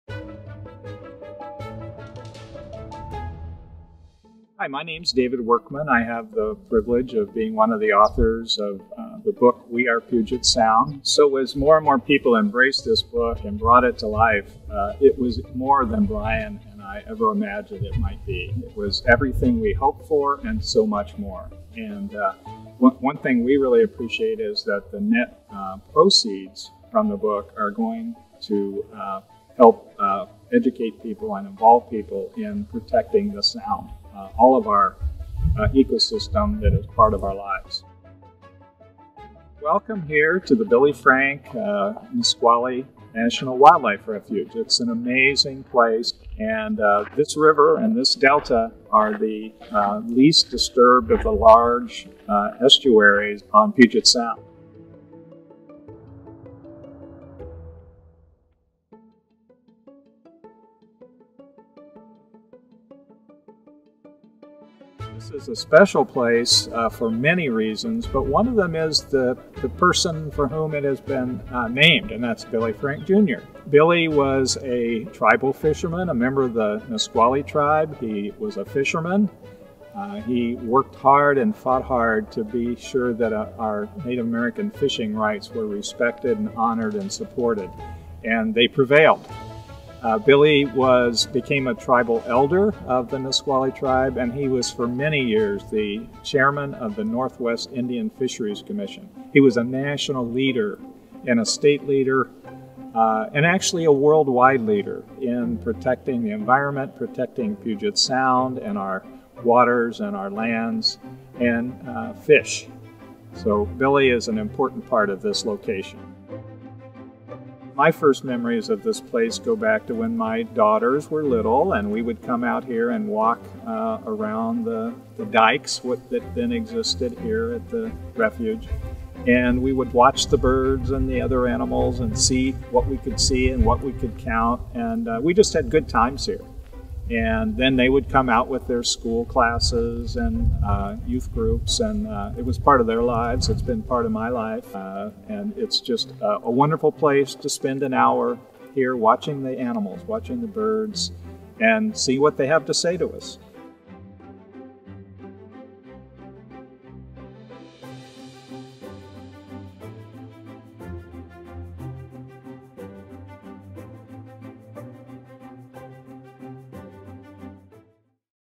David Workman: Joys of Experiencing Puget Sound
Dave Workman is a co-author of We Are Puget Sound, Discovering and recovering the Salish Sea - a multimedia book and advocacy campaign that amplifies the voices and ideas working to protect and restore Puget Sound. Listen to first accounts of his connection that formed his love and appreciation of Puget Sound. It all started at the Billy Frank Jr. Nisqually National Wildlife Life refuge named in honor of Nisqually tribal leader and treaty rights activist Billy Frank, Jr.

Credits: Executive Produced by Zachary Pullin, Communications Director and Rein Attemann of Washington Environmental Council, with support from Braided River. Special gratitude to the James M. Lea Foundation for their gracious support of this We Are Puget Sound video series.